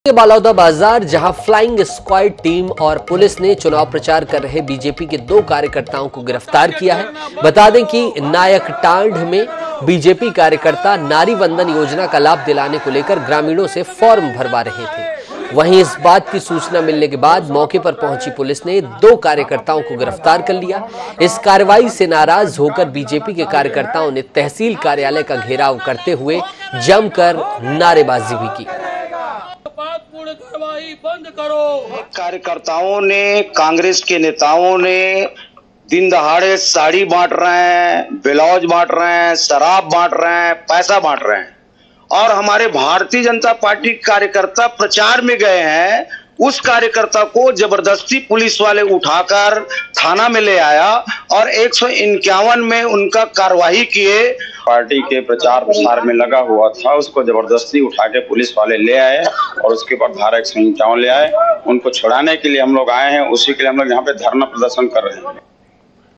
Flying Squad और Police Team की कार्रवाई | BJP के दो कार्यकर्ताओं को किया गिरफ्तार
INH, Flying Squad और Police Team की कार्रवाई | BJP के दो कार्यकर्ताओं को किया गिरफ्तार

Incoming Search Terms: